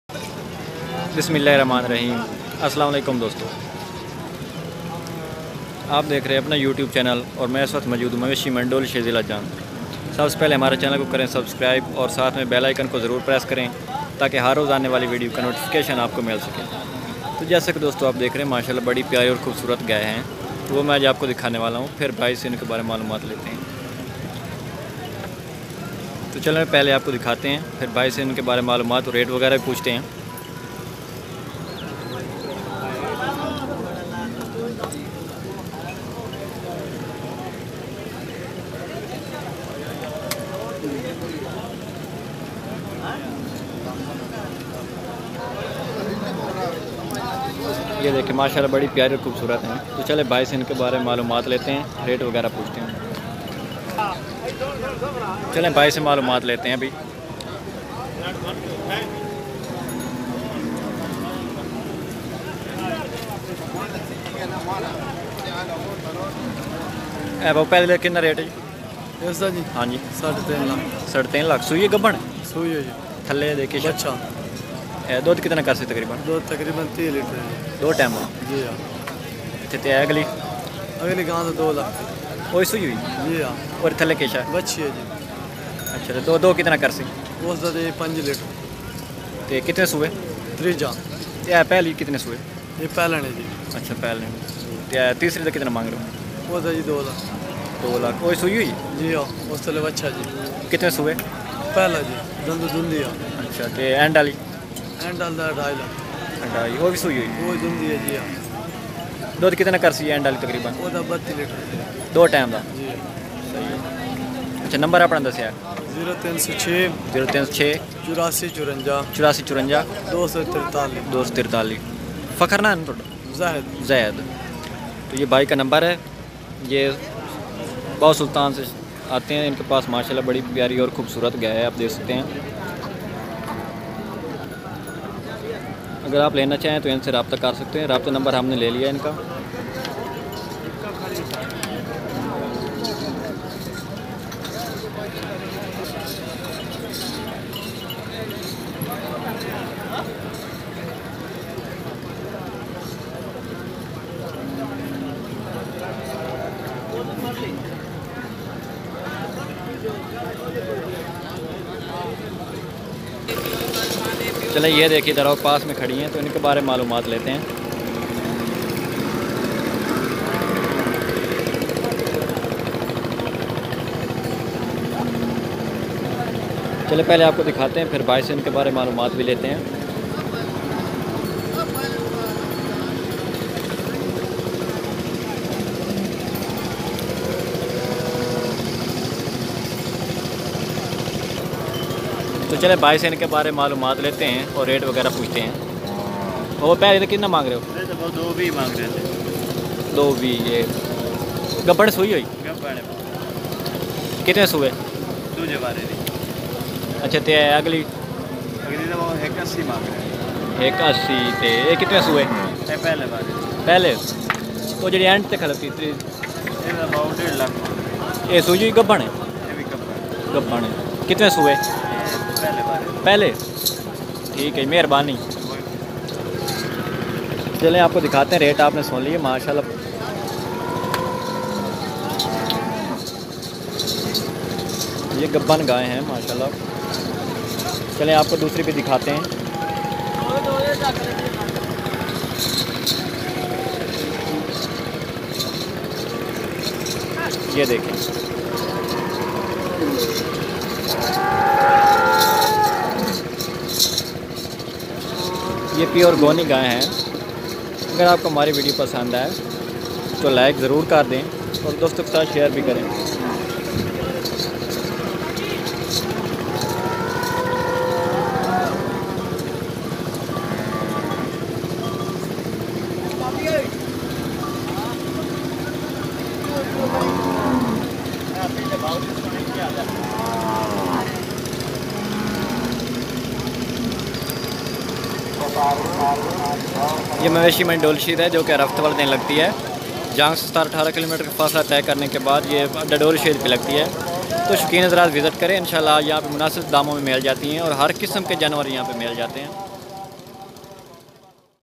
Australian Frezian |Walayti Cross Cows For Sale In Janwar Mandi Doli Shaeed Jhang
Waseem sargana